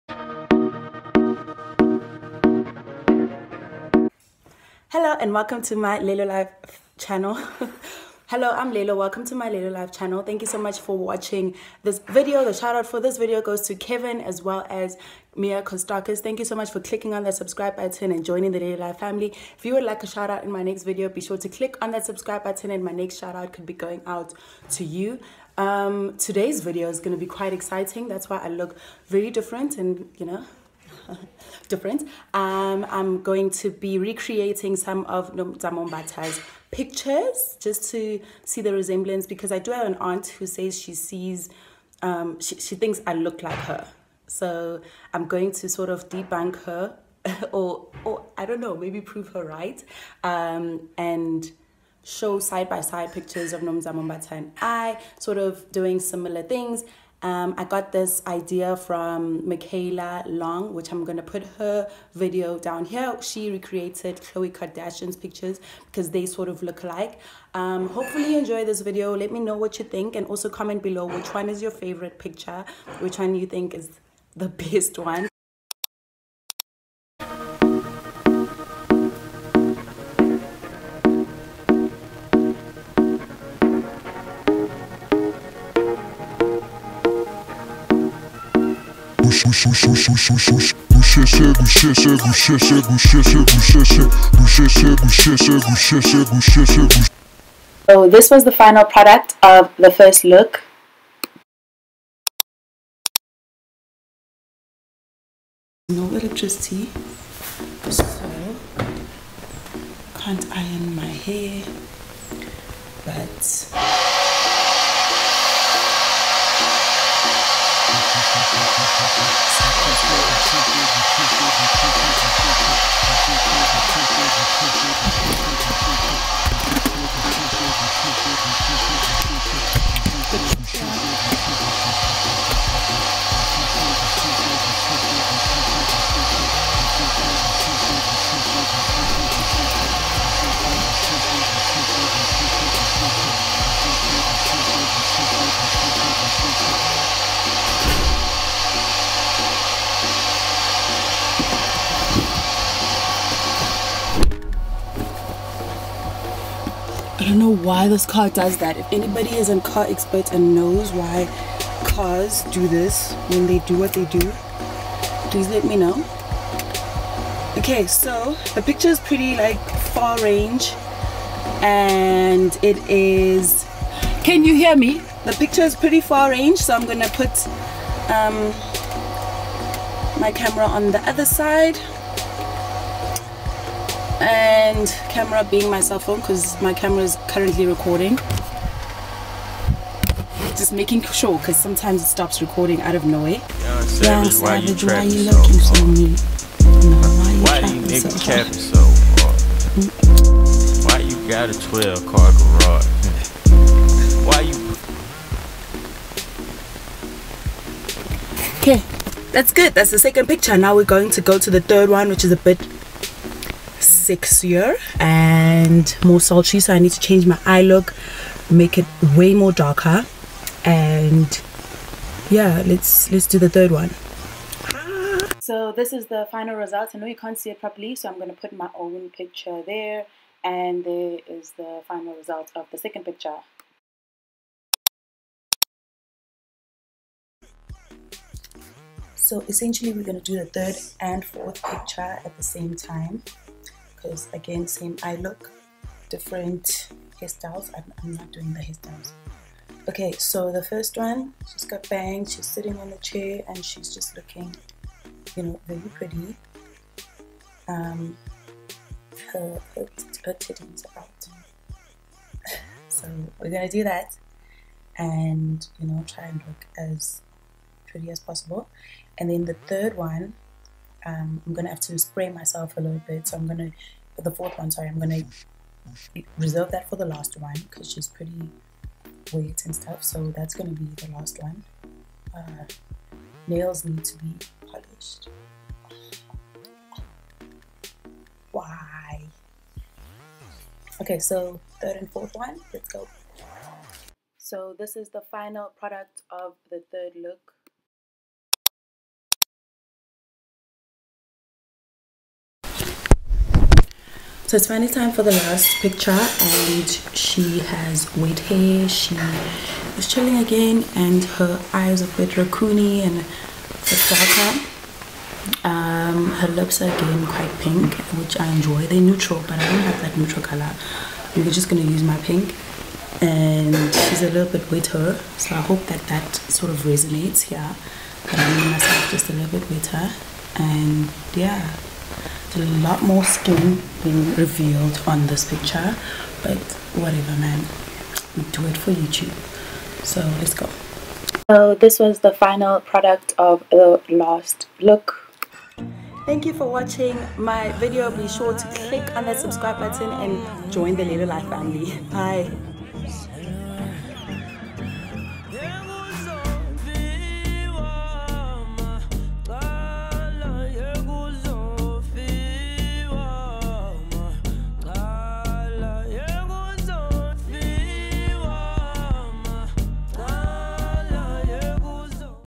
I TRIED RECREATING NOMZAMO MBATHA’S PHOTOS | SOUTH AFRICAN YOUTUBER
*WATCH ME*
Inspired by Mikaela Long recreating Khloe Kardashians pictures I decided to attempt something similar with a South African twist
Let me know if you think there’s a resemblance and which one is your favourite look.

@lelobetweetin on Twitter 😊